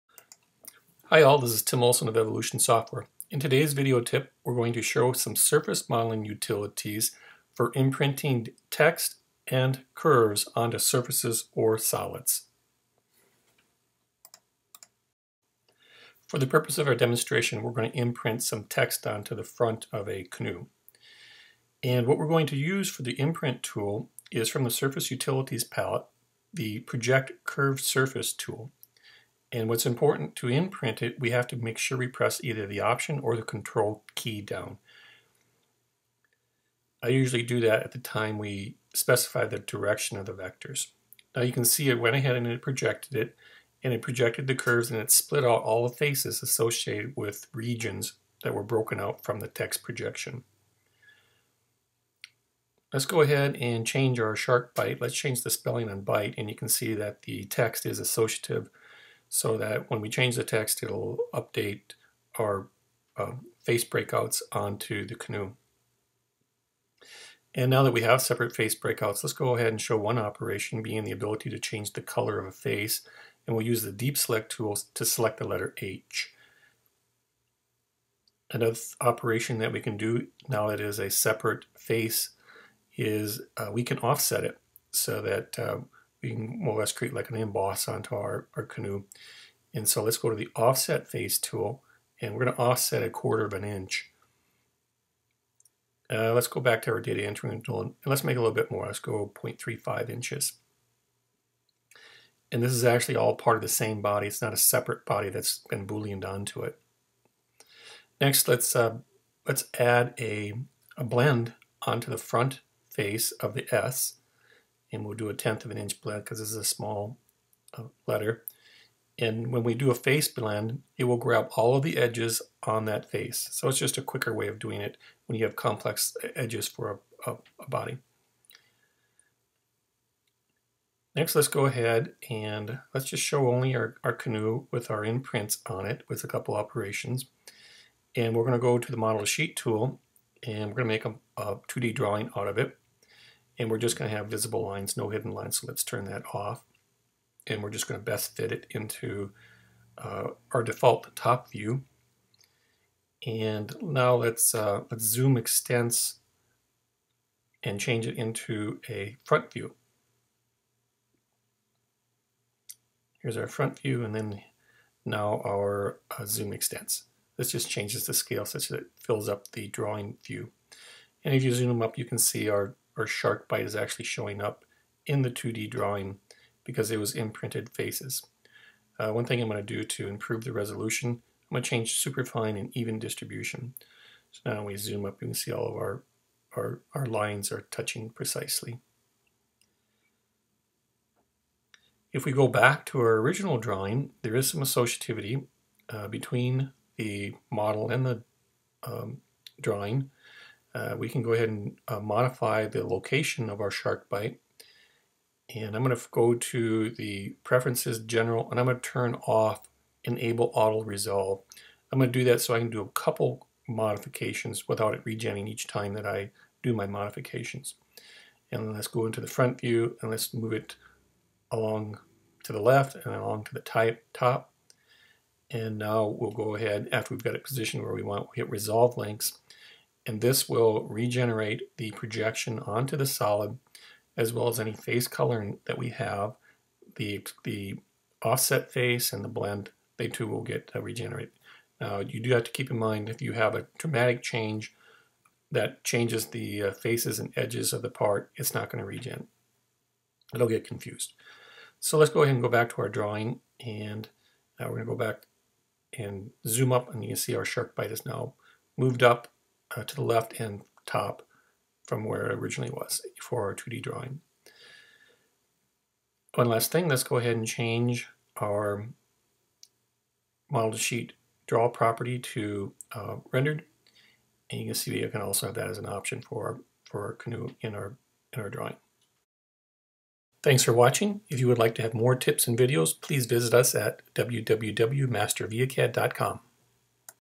Imprinting Text with ViaCAD & Shark
Imprints text onto a solid suitable for face offsets and color attributes.